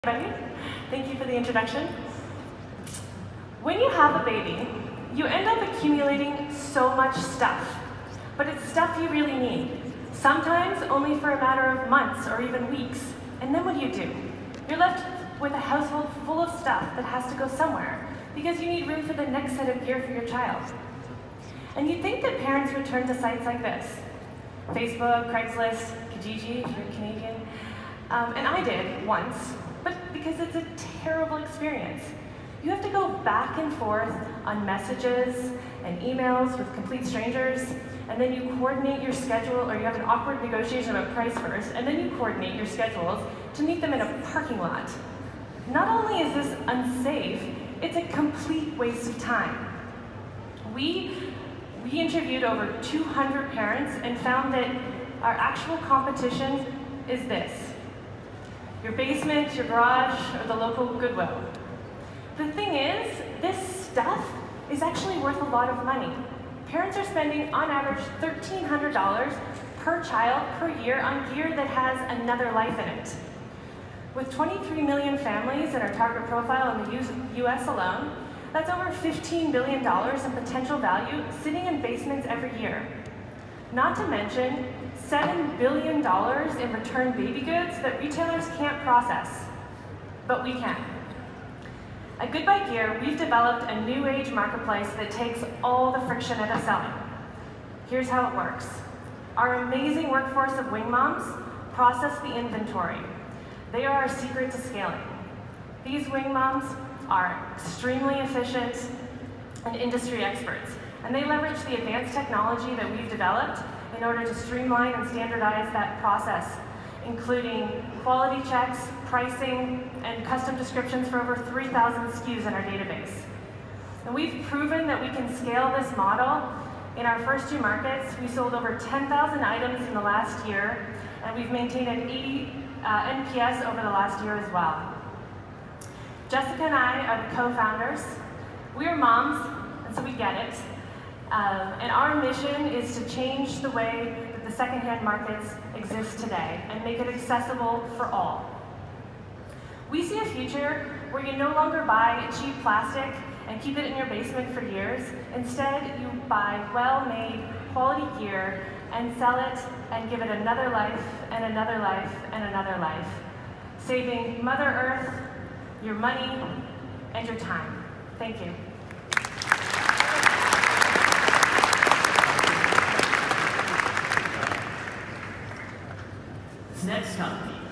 Good Buy Gear at the Collision Conference
BoomtownAlum Kristin Langenfeld took Good Buy Gear’s mission to Toronto for the Collision Conference. Hear how Good Buy Gear is changing the second-hand industry.